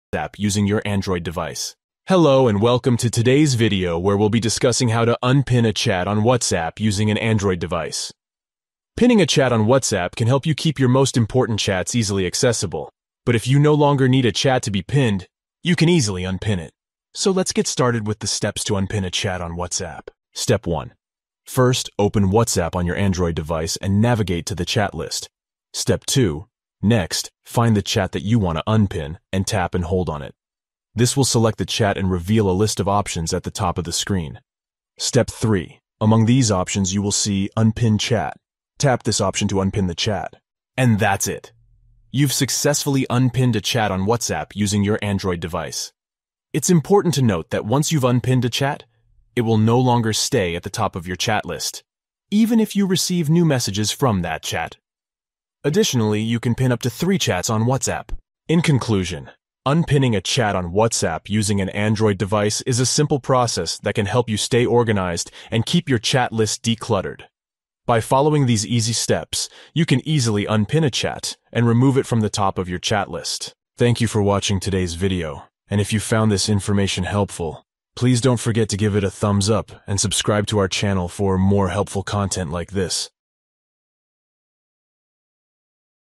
How To Unpin A Chat On Whatsapp On Android
In this video, we'll show you how to unpin a chat on WhatsApp on your Android phone. By following these simple steps, you can remove a chat from the pinned section or organize your chats differently.

Step 1: Tap and hold the pinned chat that you want to unpin.
Step 2: Tap on "Unpin chat". This will remove the chat from the pinned section.

That's it! By following these easy steps, you can unpin a chat on WhatsApp on your Android phone. Remember, you can always pin a chat back by tapping and holding it and then tapping on "Pin chat".